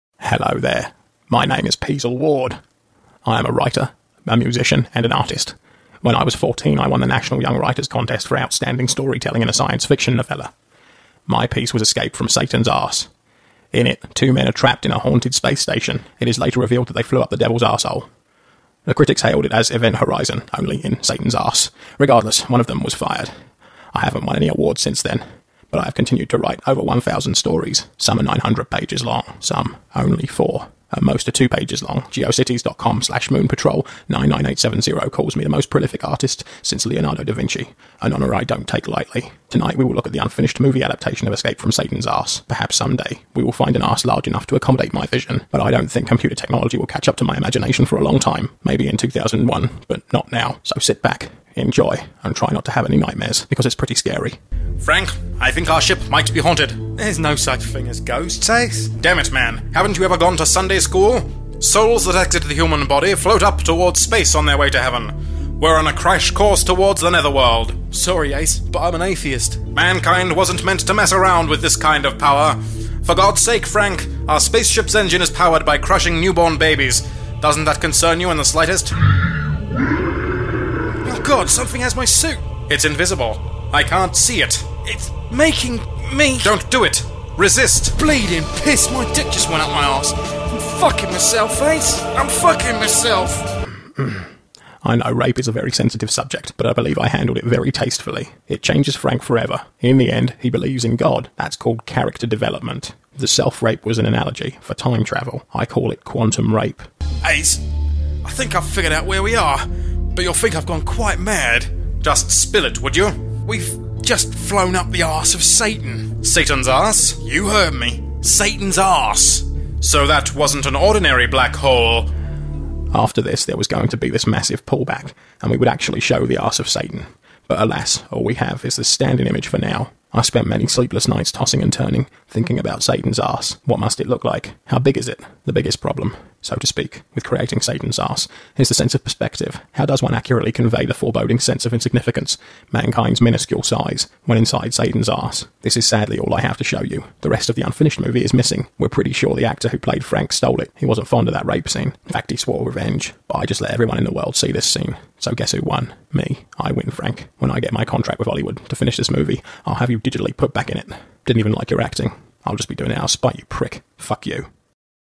Peezle Ward's "Escape From Satan's Ass Hole"
Peezle Ward's unfinished, first attempt to create a thrilling science fiction adventure regarding flying a spaceship up Satan's Ass Hole.

We're unpopular and don't make a dime from Youtube ads!